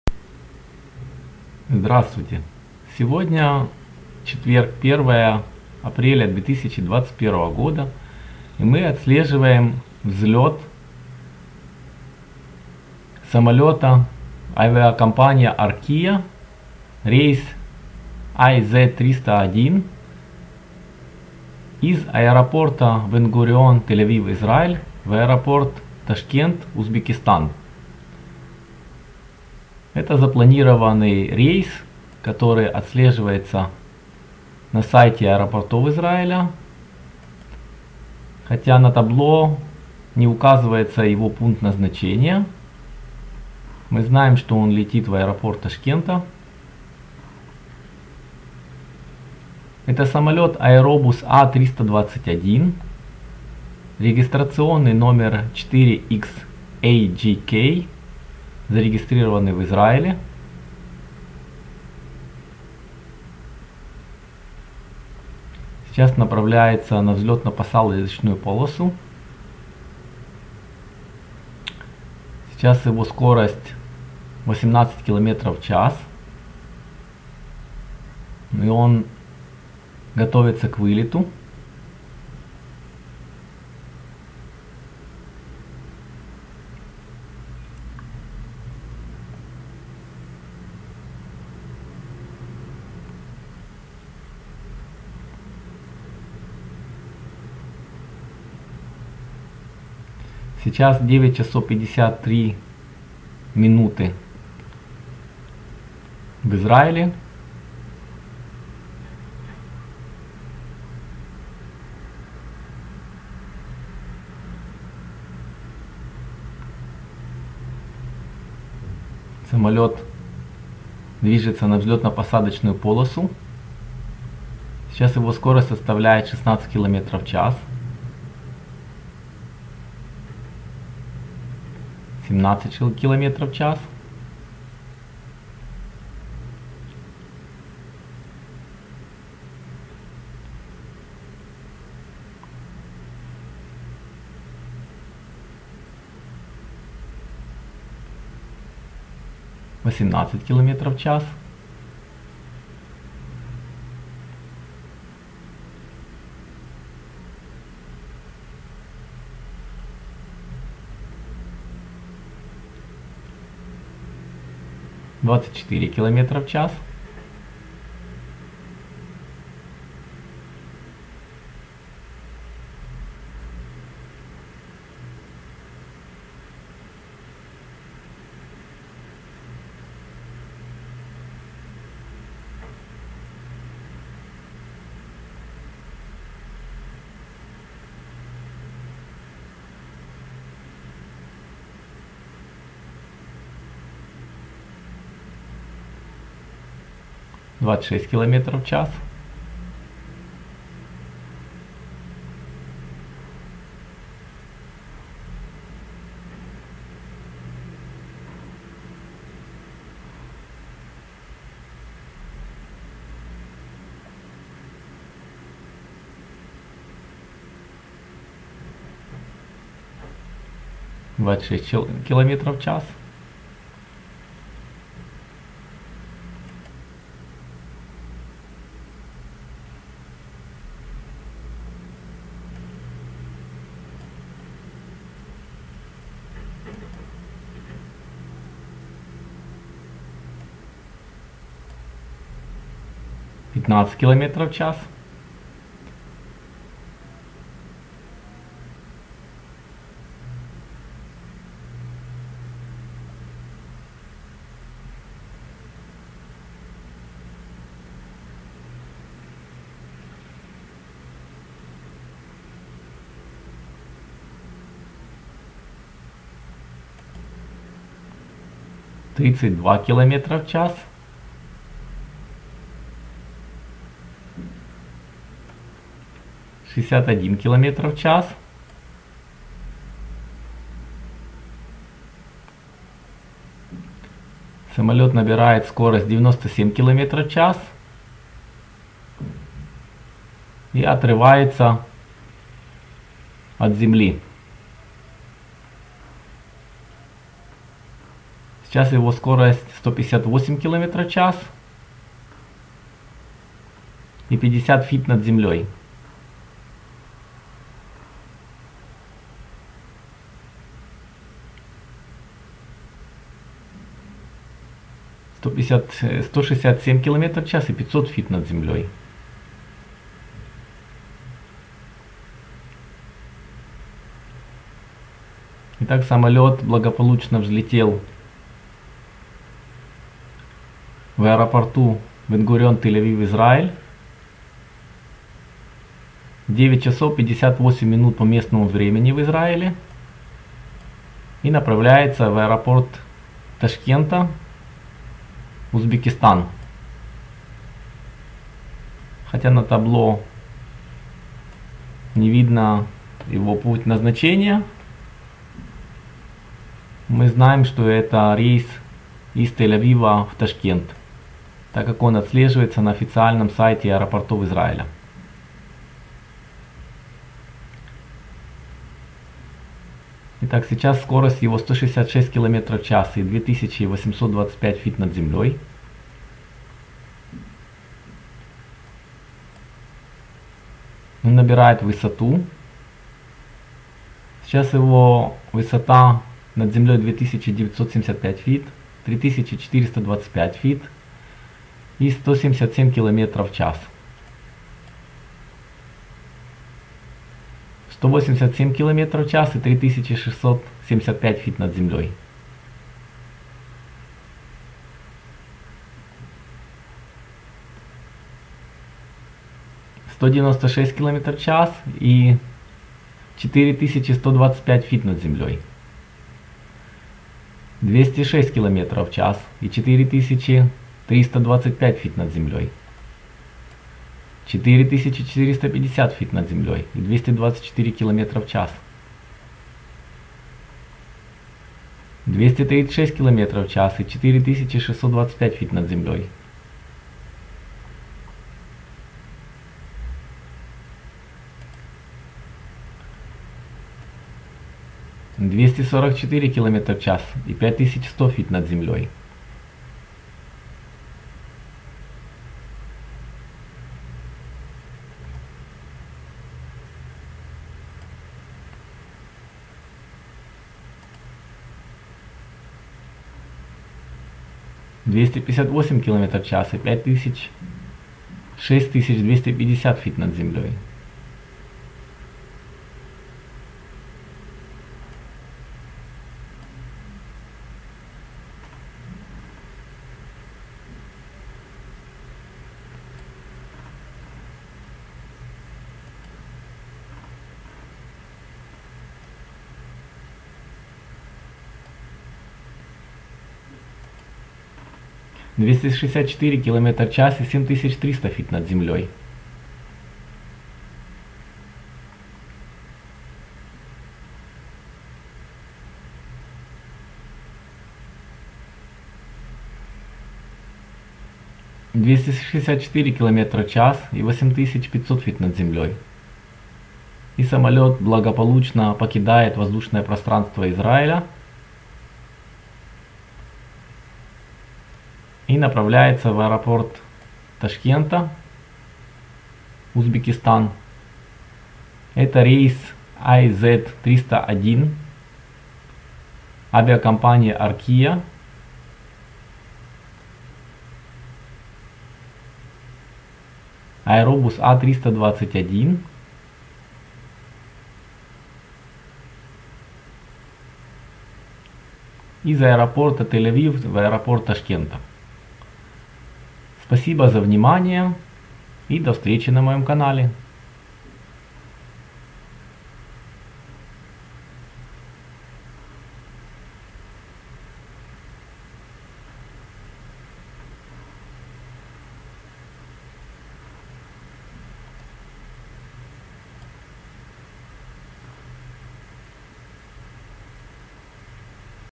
Рейс IZ301 из Тель Авива (TLV) в Ташкент (TAS).1.04.21
Рейс IZ301 авиакомпании ARKIA из Бен Гурион (TLV) Израиль в Ташкент (TAS) Узбекистан 1 апреля 2021 года. Самолёт Airbus A321.